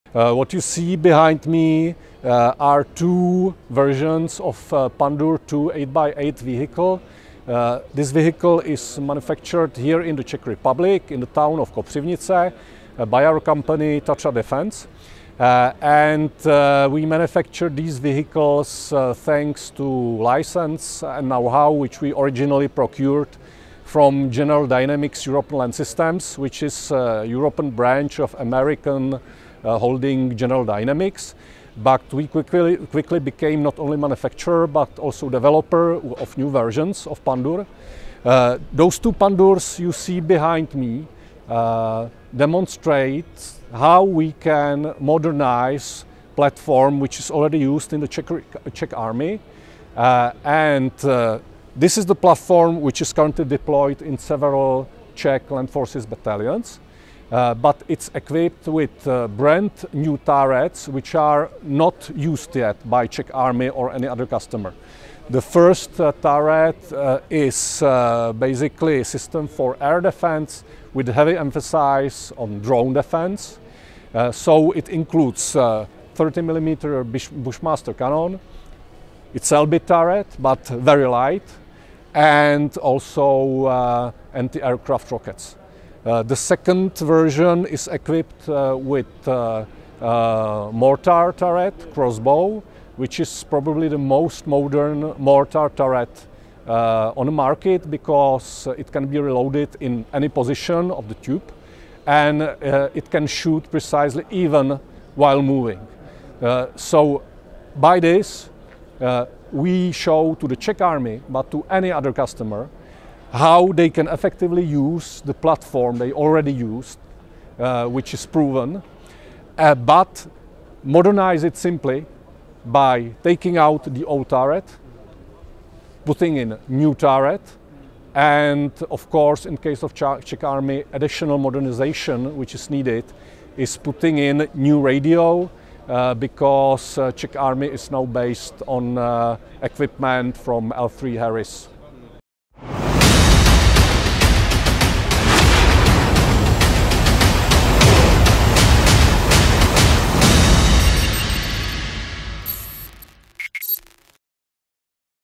From air defense to mortar systems: The modular capabilities of Pandur II 8x8
The Pandur II 8x8 is a well-established armoured wheeled platform forming a key part of the Czech Army’s ground forces and serving with several international operators. Developed and produced by CSG Group's Tatra Defence Vehicle (TDV) under licence from General Dynamics European Land Systems, the vehicle is recognised for its modular design, allowing multiple mission configurations and straightforward modernisation.

In an interview with defensehere.com, CSG Group spokesperson Andrej Čírtek discussed two upgraded variants of the Pandur II 8x8 recently configured to demonstrate the platform’s adaptability. One variant integrates an air defence system featuring a 30 mm Bushmaster cannon optimised for counter-drone roles, supported by anti-aircraft missiles. The other incorporates the 120mm Crossbow mortar turret, capable of reloading in any tube position and delivering precise fire while on the move.

The vehicle’s welded self-supporting hull offers ballistic and mine protection up to STANAG 4569 Level 4, along with CBRN protection systems. Power is provided by a Cummins ISLe diesel engine coupled with a ZF automatic transmission, enabling a maximum speed of 105 km/h and a range of up to 700 km. The Pandur II 8x8 is also transportable by standard NATO transport aircraft such as the C-130, A400M, C-5, and C-390.

With its modular architecture, the Pandur II 8x8 can be configured for roles including infantry fighting vehicle, armoured personnel carrier, fire support vehicle, command post, ambulance, reconnaissance vehicle, and self-propelled mortar. This approach enables users to modernise existing fleets without replacing the entire platform, extending operational life and enhancing capability.

TDV remains the exclusive manufacturer and service provider for Pandur II 8x8 vehicles in the Czech Army and selected territories worldwide.